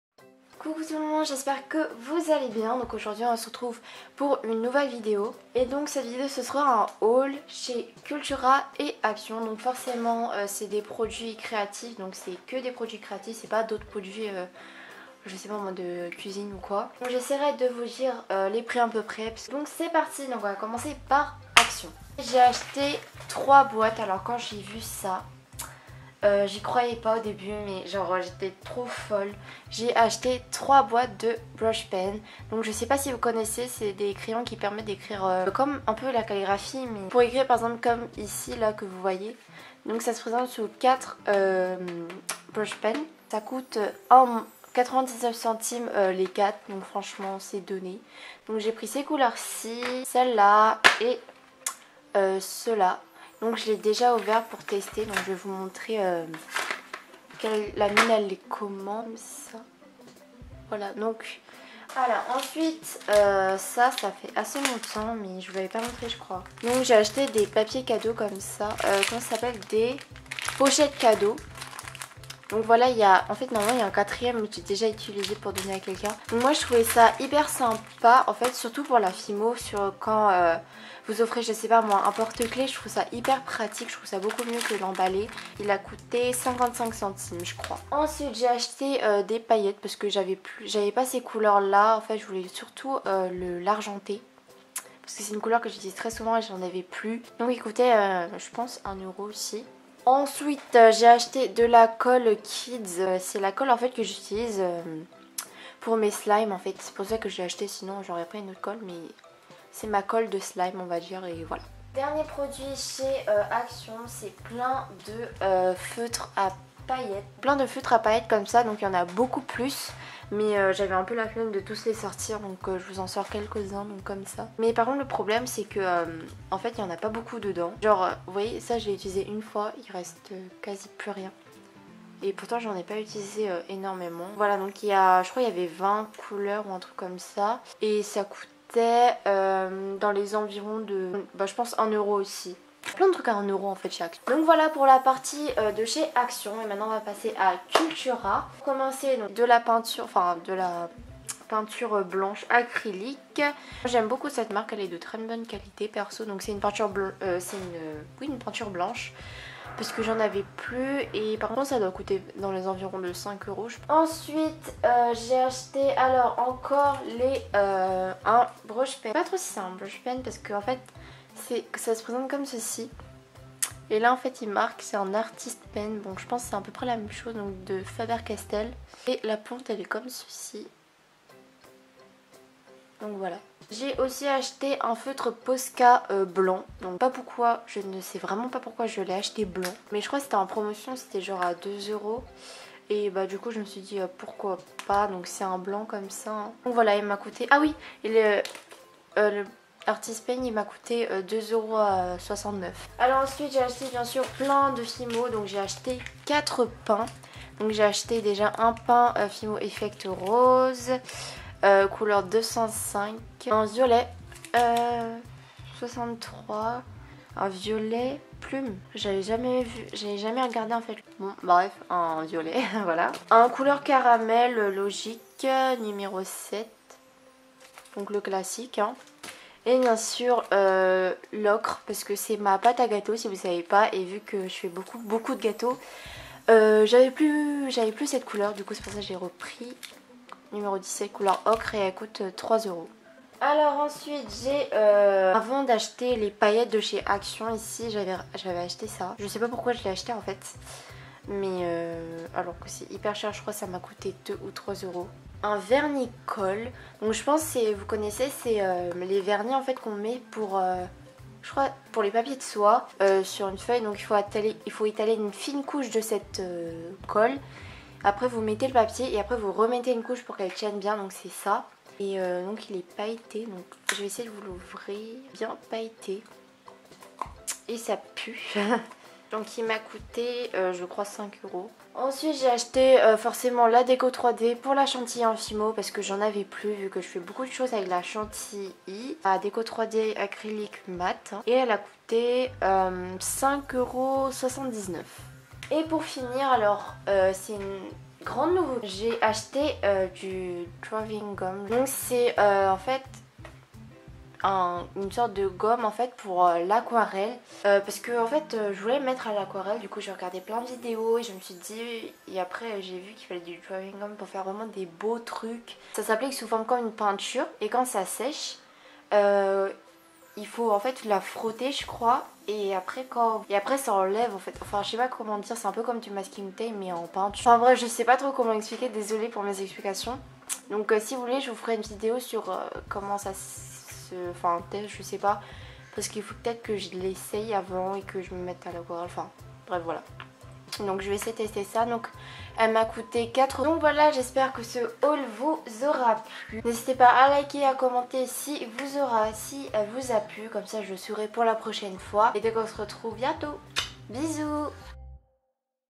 HAUL CRÉATIF | ACTION ET CULTURA ♡
♡ Déroule ♡

YEEEEEEH ! J'ai fait pas mal d'achat depuis et je voulais vous montrer les produits que j'ai acheté à Cultura et à Action !

Je monte avec Vegas Movie Studio
Je monte les photos avec Picmonkey

😘 Bisous mes cookies 😘